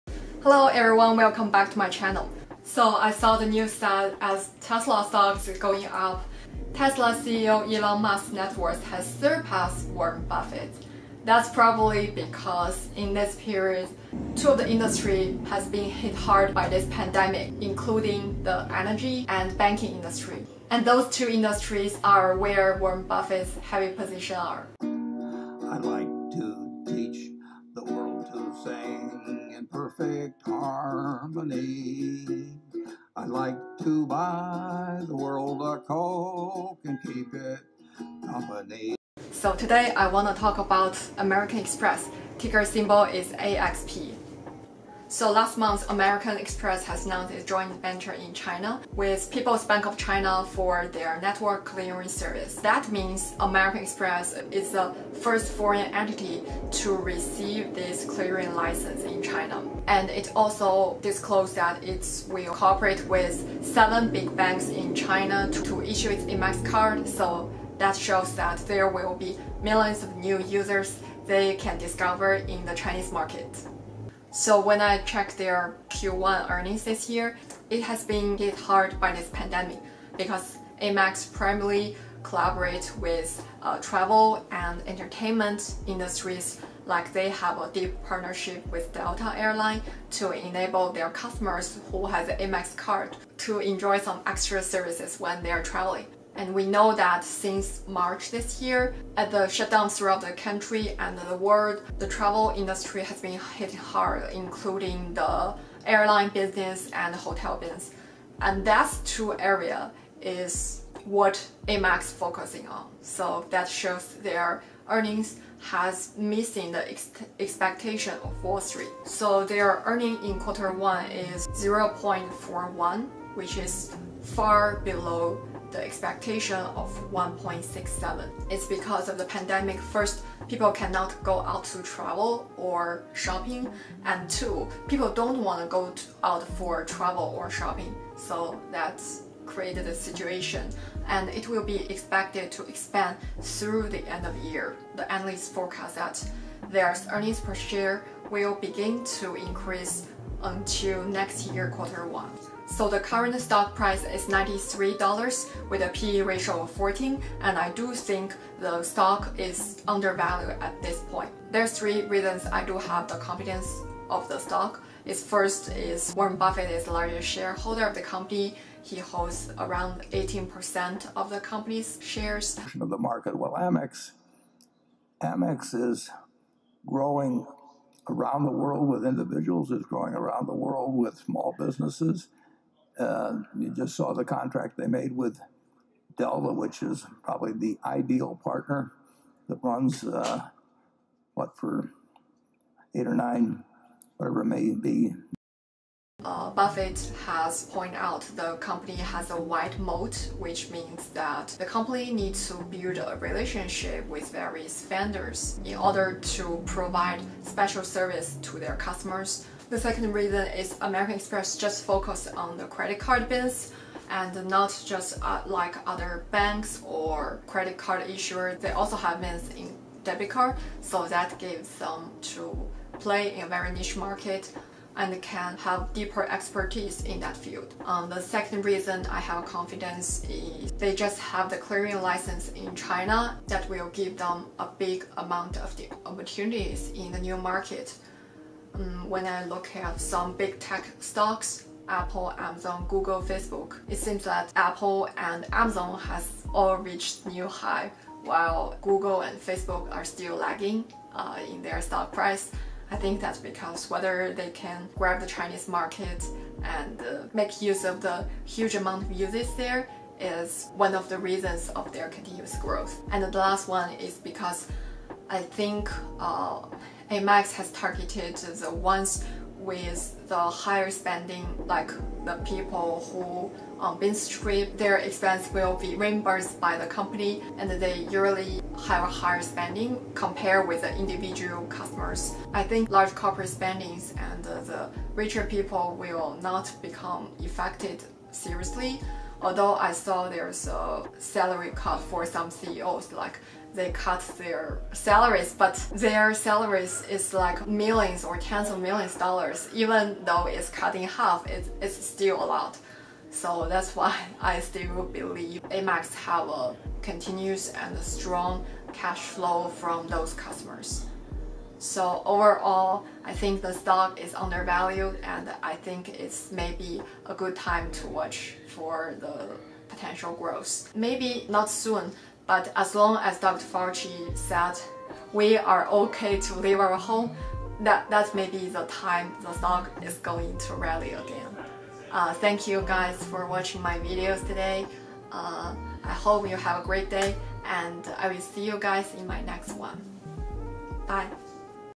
Is American Express Stock a Good Buy at $93? ♥️♠️♦️♣️ | New Clearing License in China!
American Express (NYSE: AXP) announced in June 13th that its joint-venture in mainland China, Express (Hangzhou) Technology Services Company Limited (“Express Company”), has received approval from the People’s Bank of China (PBOC) for a network clearing license. With this, American Express becomes the first foreign payments network to be licensed to clear RMB transactions in mainland China. The company expects to begin processing transactions later this year.

Check out the awesome stock screener App:
Android Version